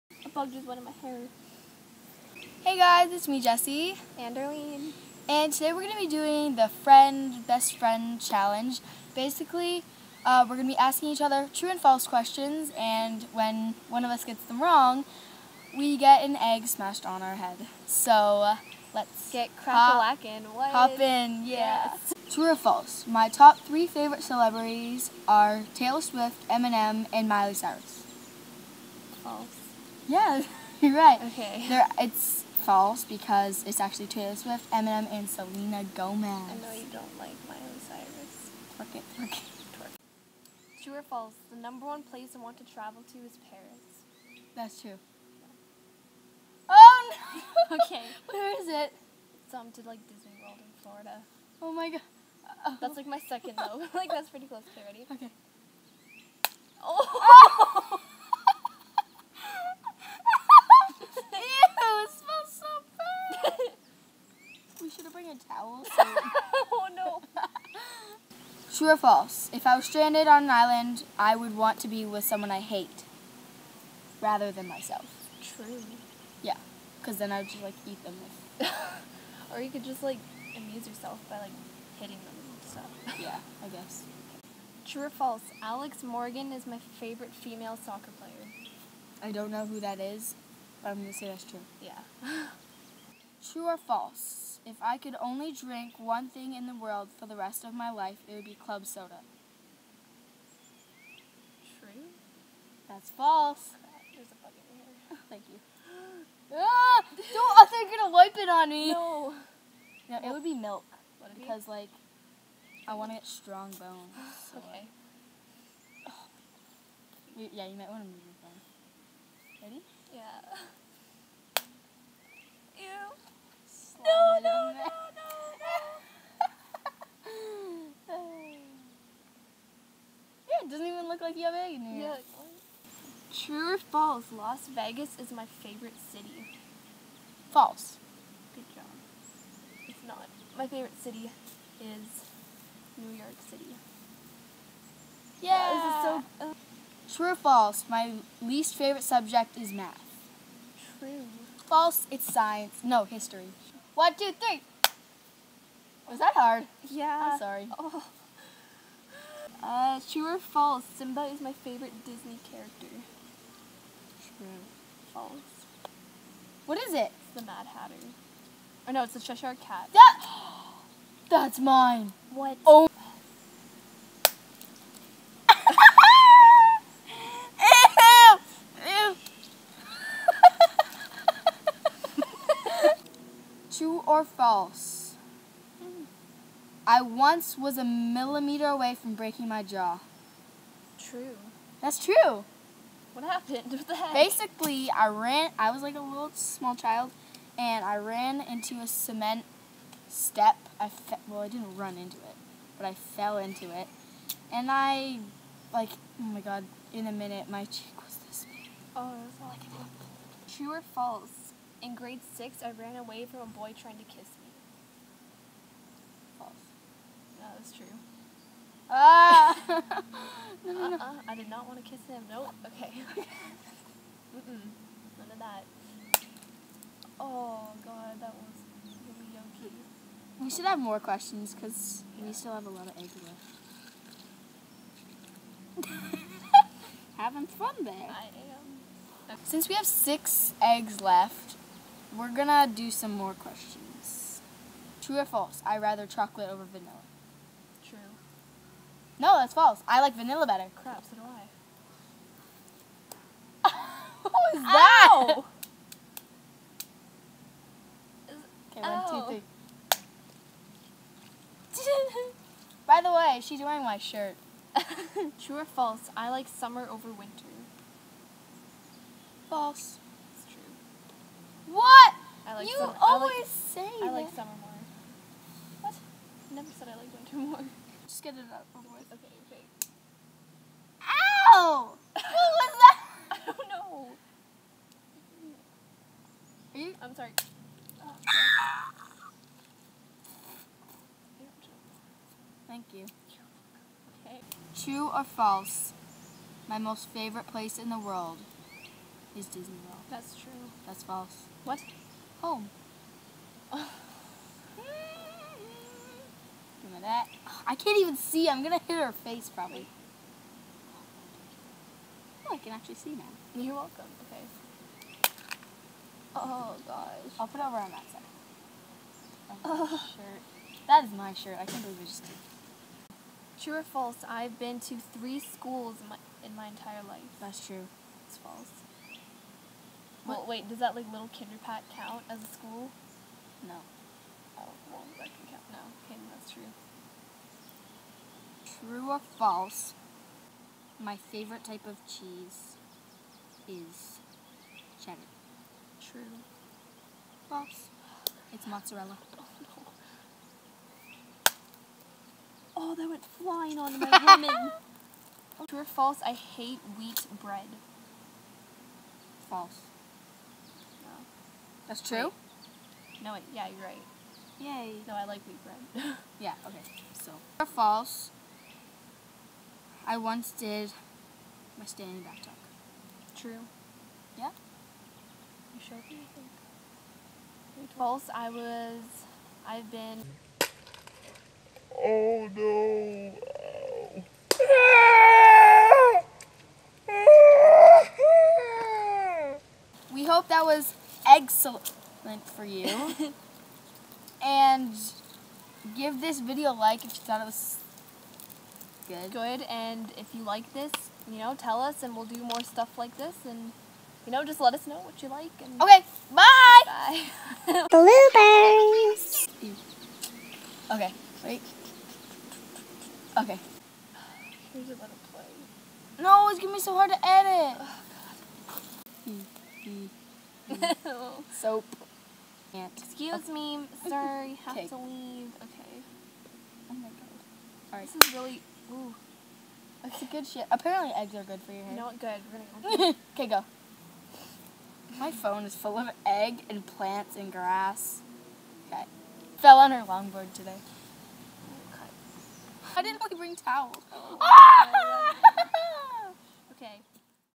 Friend Challenge (egg edition) ft:Darlene
Special thanks to Darlene for doing this video with me... And willing to get bathed in egg ;)
We both had so much fun making this video and if you enjoy these types of videos. Let me know in the comments and give this video a like.. It really helps!!
Thank you all for 6.5k!